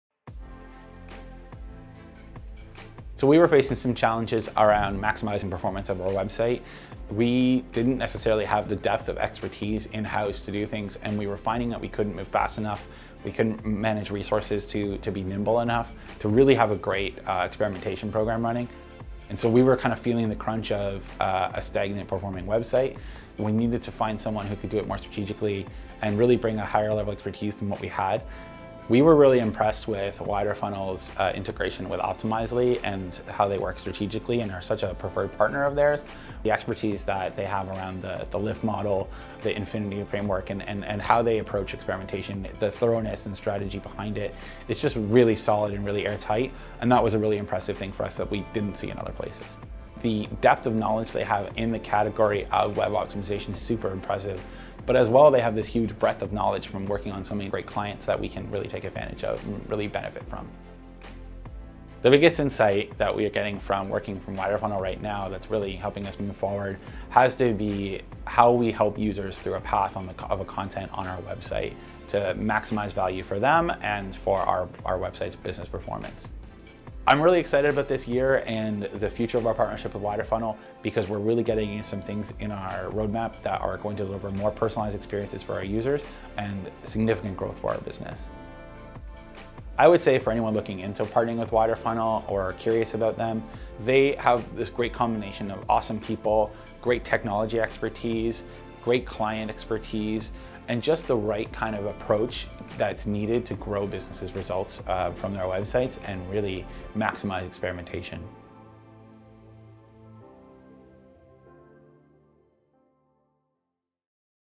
FreshBooks - Widerfunnel Client Success Story
Freshbooks is a cloud accounting solution designed for small business owners. Creating a meaningful customer experience is essential for Freshbooks to increase and retain subscribers to grow their business.